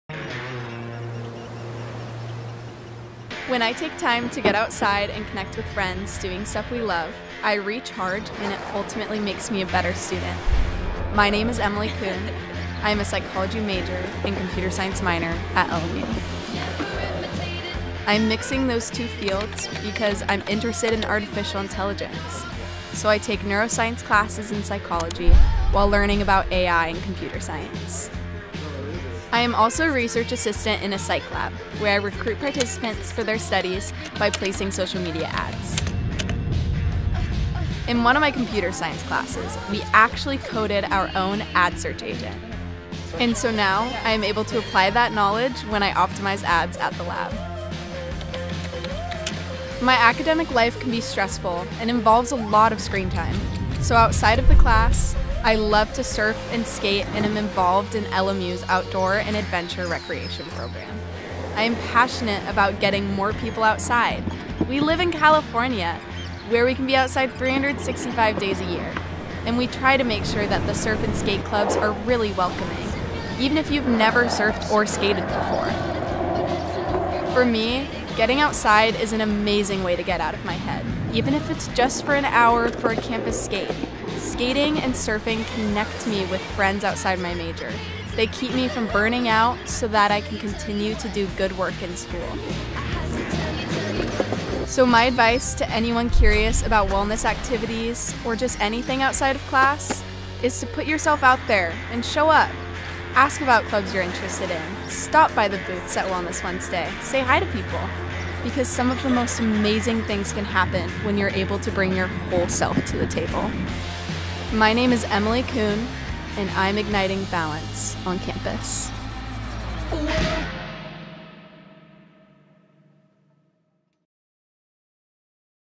Ignite Balance: Emilie Coon '24
Emilie Coon ’24 studies neuroscience and artificial intelligence. It’s a challenging course load, and she finds balance in LMU’s surf and skate communities.